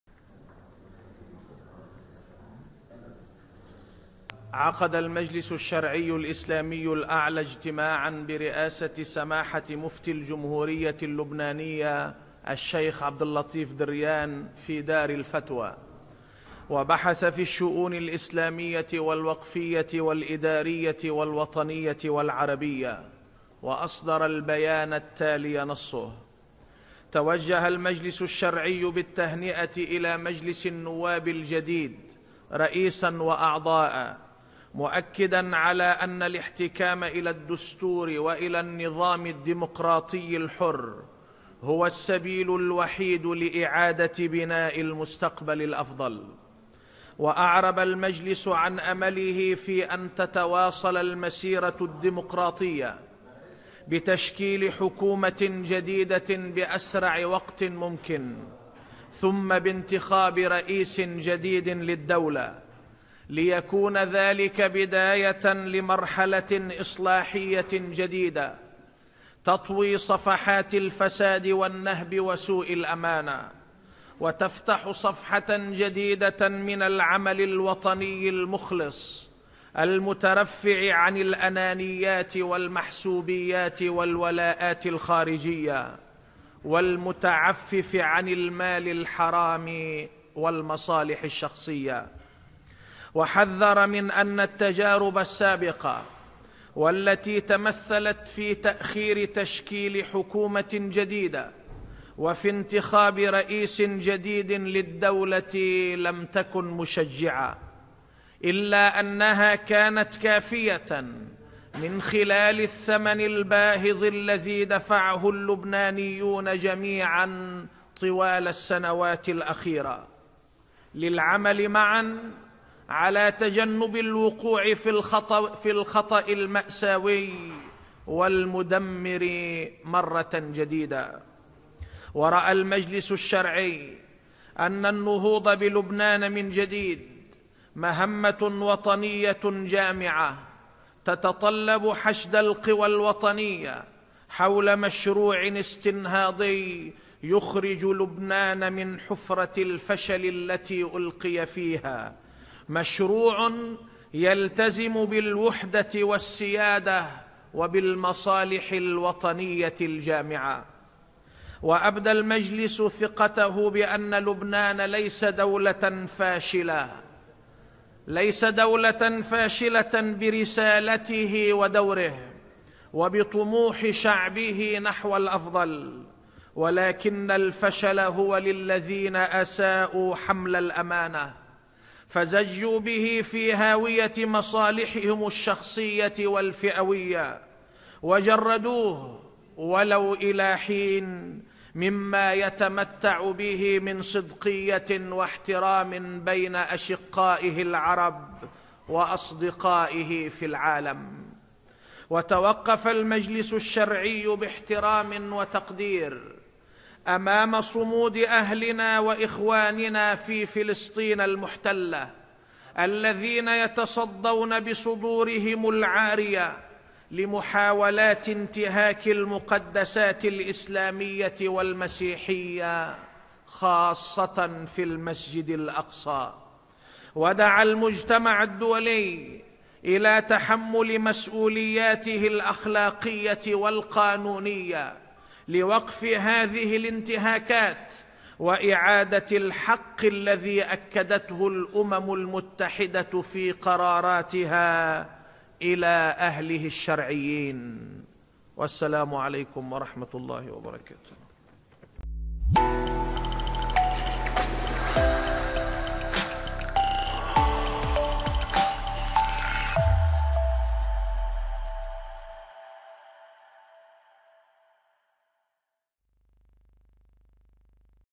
المجلس الشرعي: نأمل أن تتواصل المسيرة الديمقراطية بتشكيل حكومة جديدة بأسرع وقت
توجه المجلس الشرعي الإسلامي الأعلى، في بيان أصدره بعد اجتماع في دار الفتوى برئاسة مفتي الجمهورية الشيخ عبد اللطيف دريان، بالتهنئة الى مجلس النواب الجديد رئيسا وأعضاء، مؤكدا ان "الاحتكام الى الدستور والى النظام الديمقراطي الحر هو السبيل الوحيد لإعادة بناء المستقبل الأفضل".

وأعرب المجلس عن أمله في "أن تتواصل المسيرة الديمقراطية بتشكيل حكومة جديدة بأسرع وقت ممكن، ثم بانتخاب رئيس جديد للدولة، ليكون ذلك بداية لمرحلة إصلاحية جديدة تطوي صفحات الفساد والنهب وسوء الأمانة، وتفتح صفحة جديدة من العمل الوطني المخلص المترفع عن الأنانيات والمحسوبيات والولاءات الخارجية، والمتعفف عن المال الحرام والمصالح الشخصية".

وحذر من أن "التجارب السابقة، والتي تمثلت في تأخير تشكيل حكومة جديدة وفي انتخاب رئيس جديد للدولة، لم تكن مشجعة، إلا أنها كانت كافية من خلال الثمن الباهظ الذي دفعه اللبنانيون جميعا طوال السنوات الأخيرة، للعمل معا على تجنب الوقوع في الخطأ المأساوي والمدمر مرة جديدة".

ورأى المجلس الشرعي ان "النهوض بلبنان من جديد، مهمة وطنية جامعة تتطلب حشد القوى الوطنية حول مشروع استنهاضي يخرج لبنان من حفرة الفشل التي ألقيَ فيها، مشروع يلتزم بالوحدة والسيادة وبالمصالح الوطنية الجامعة".

وأبدى ثقته بأن "لبنان ليس دولة فاشلة برسالته ودوره، وبطموح شعبه نحو الأفضل، ولكن الفشل هو للذين أساؤوا حمل الأمانة، فزجوا به في هاوية مصالحهم الشخصية والفئوية، وجردوه- ولو الى حين- مما يتمتع به من صدقية واحترام بين أشقائه العرب، وأصدقائه في العالم".

وتوقف المجلس "باحترام وتقدير أمام صمود أهلنا وإخواننا في فلسطين المحتلة الذين يتصدون بصدورهم العارية لمحاولات انتهاك المقدسات الإسلامية والمسيحية، بخاصة في المسجد الأقصى"، ودعا المجتمع الدولي الى "تحمل مسؤولياته الأخلاقية والقانونية لوقف هذه الانتهاكات وإعادة الحق الذي أكدته الأمم المتحدة في قراراتها الى أهله الشرعيين".